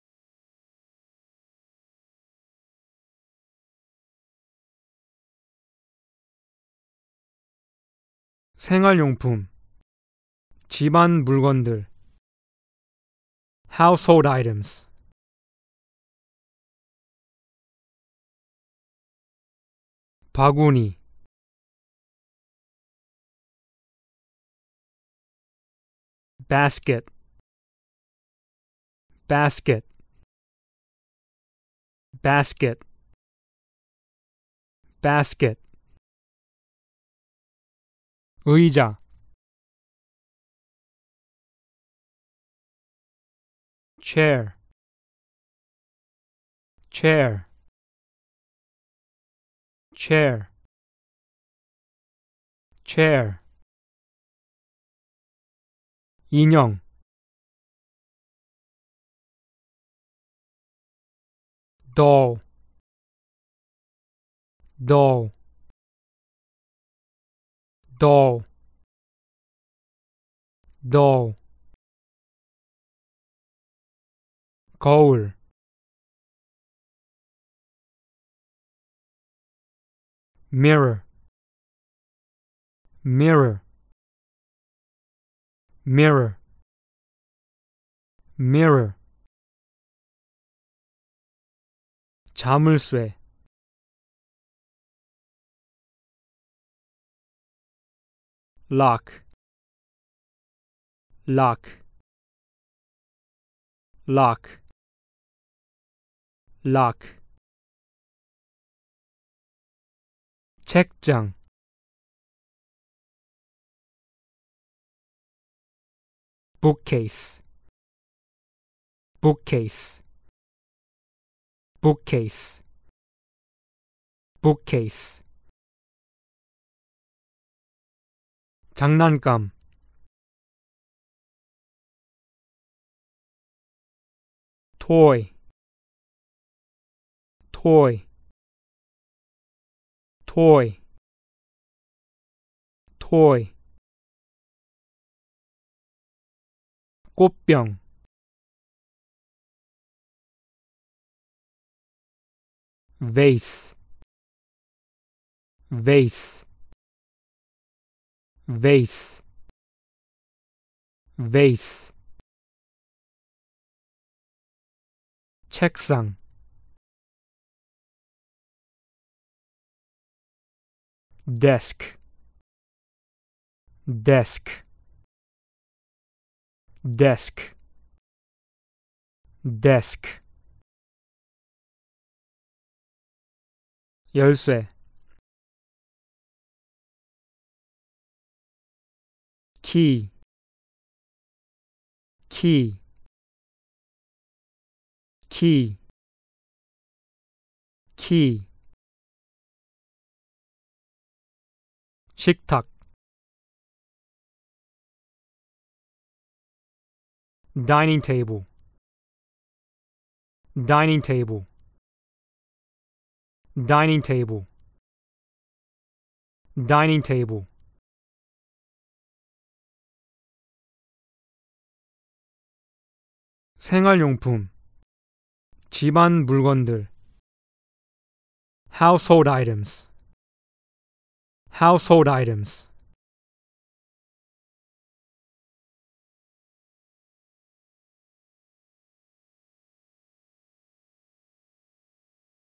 리딩 쿡 - 초등 영어단어 시험 ( Step 1 ) 주제 : 생활용품, 집안 물건들 (1)
생활용품, 집안 물건들 (1)과 관련된 영어 단어들을 제대로 학습했는지 시험을 보는 영상입니다.

"책장, 책상, 의자, 식탁, 거울, 꽃병, 자물쇠, 열쇠, 바구니, 장난감, 인형"과 같은 한국어 표현들을 영어 단어로 제대로 학습했는지 확인 해 보는 교육 영상입니다.

한국어 사용자가 영어 단어를 학습하고 암기하는 영어 교육 동영상입니다.

음성 녹음 : 오 세철 (Andrew Oh).